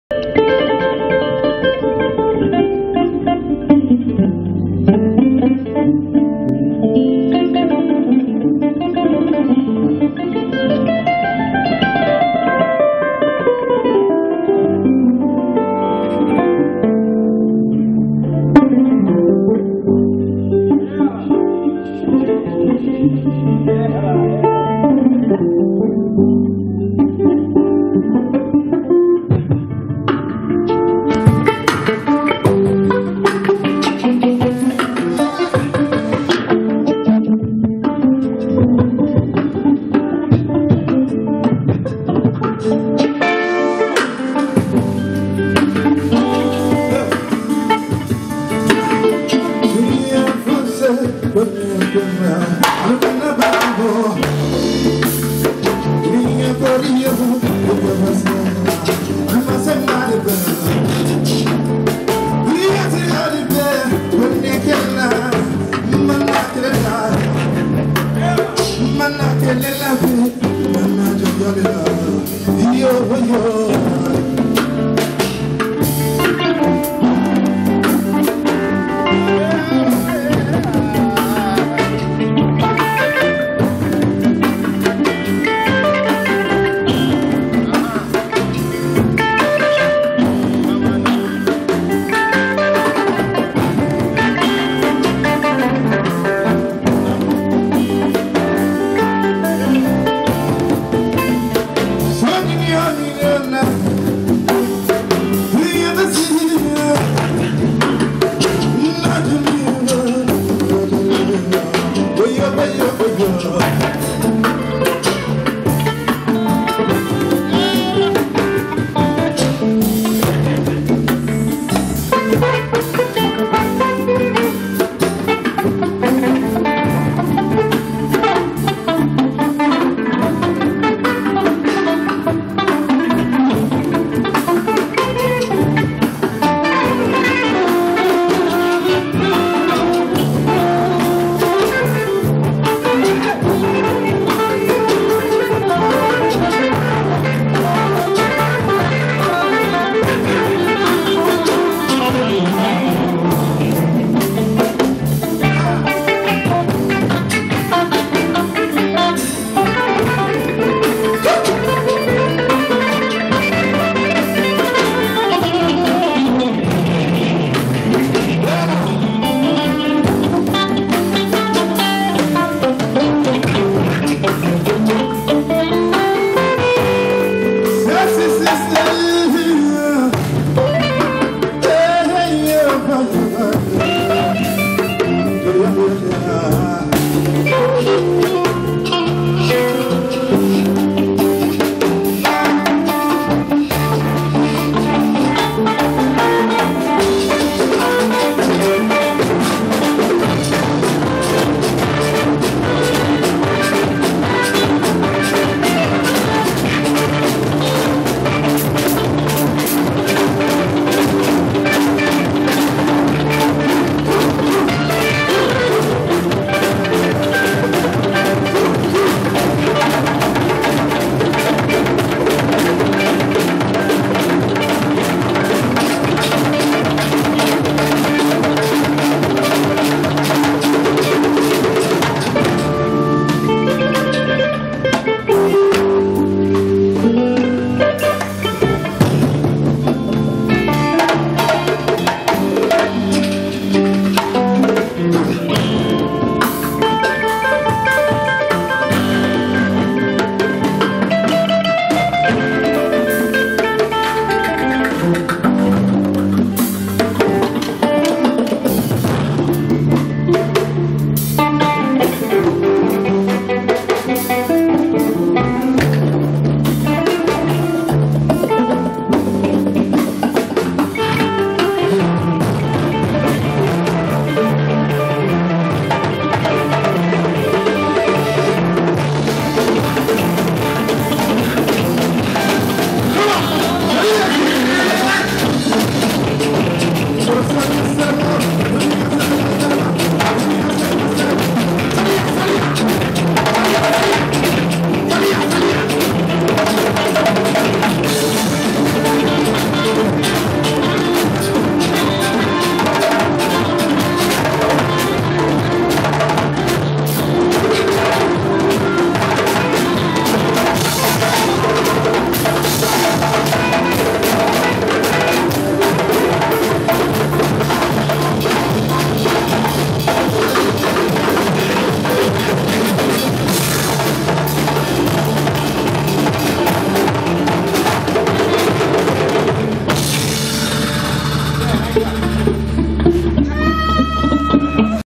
Africa Express in Marseille: Diabel Cissokho, Mamadou Sarr, Reeps One, Pauli The PSM
Africa Express rehearsal in Marseille